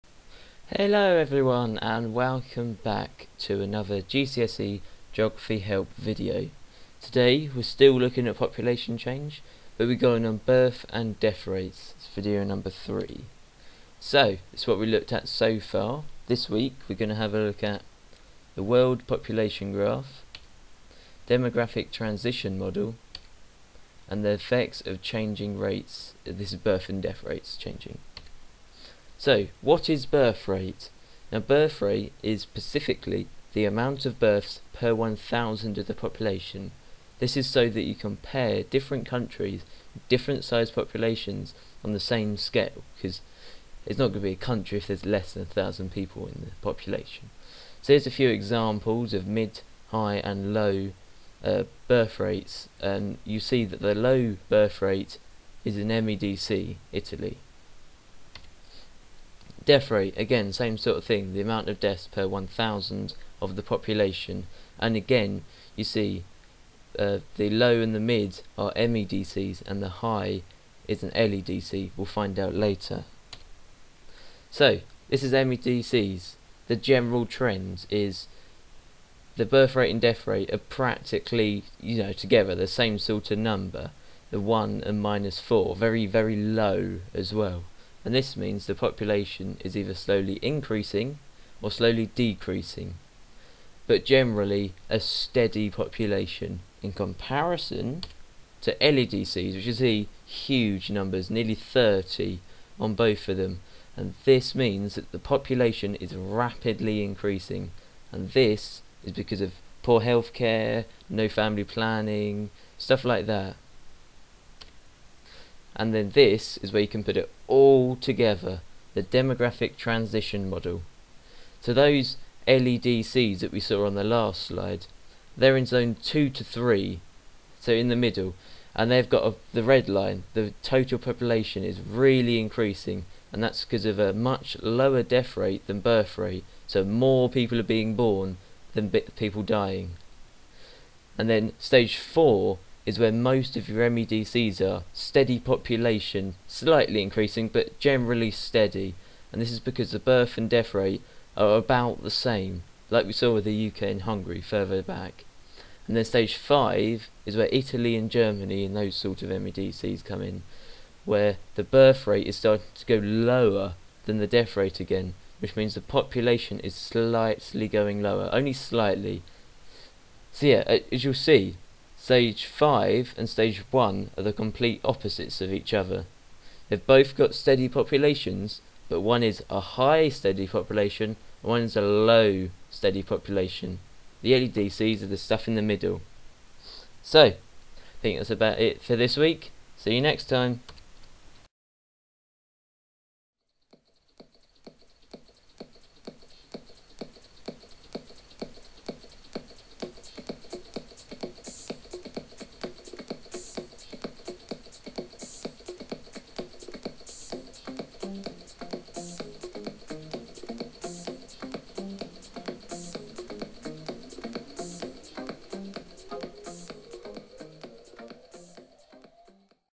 GCSE Geography Help Video 03: Birth and Death Rates
Welcome to the second series of GCSE Geography help videos. This time we are looking at population change.